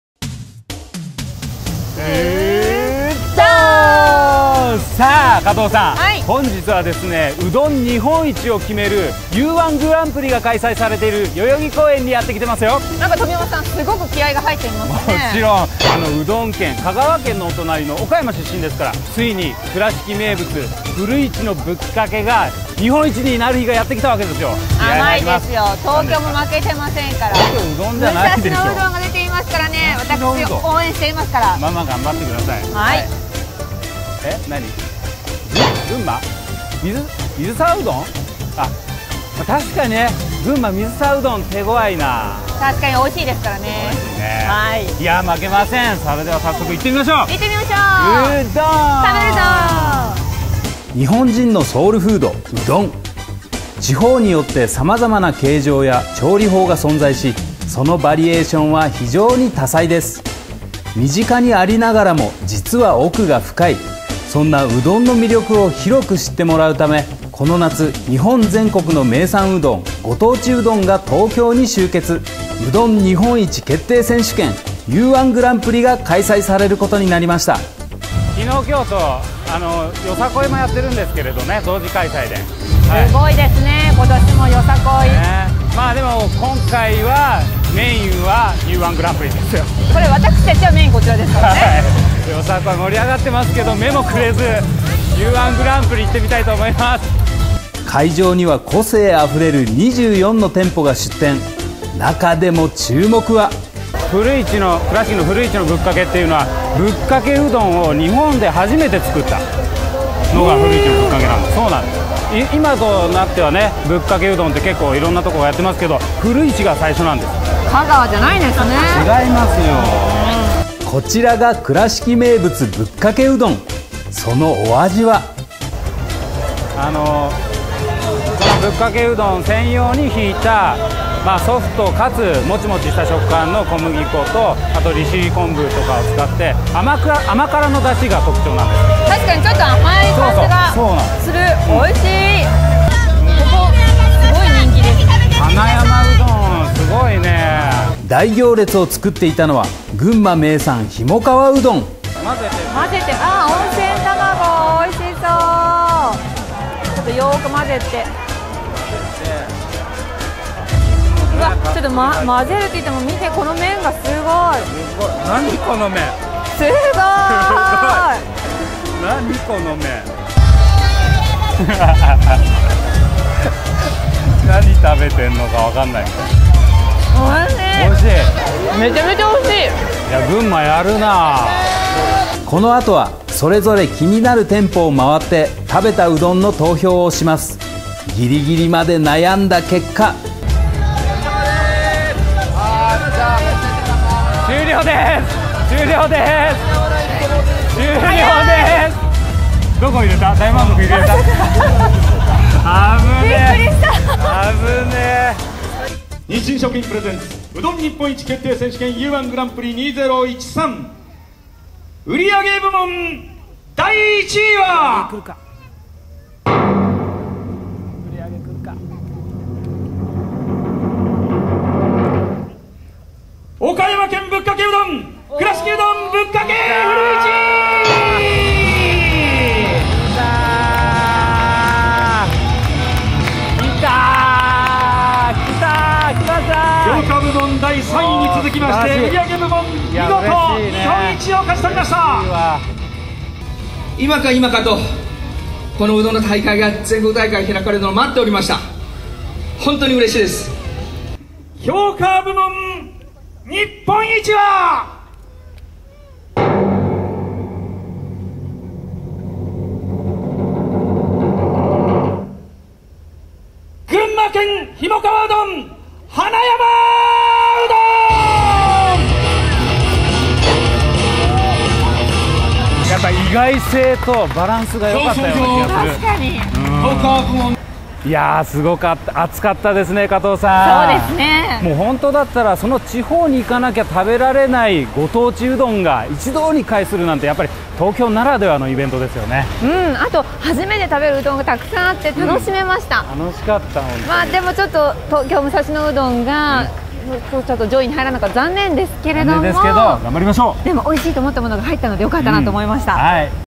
うどん日本一決定選手権U-1グランプリ2013＠代々木公園
東京の地域情報やイベントをご紹介しながら東京の住まいをご提案する住まいのナビゲーション『住まなび』。今回は表参道スーパーよさこいと同時開催された、うどん日本一決定選手権U-1グランプリ2013に行ってきました。全国のご当地うどんが、東京代々木公園に集結！日本一の栄冠に輝くのは果たして...？
様々なイベントが開催される表参道・代々木公園エリア近くの物件は、こちらから探せます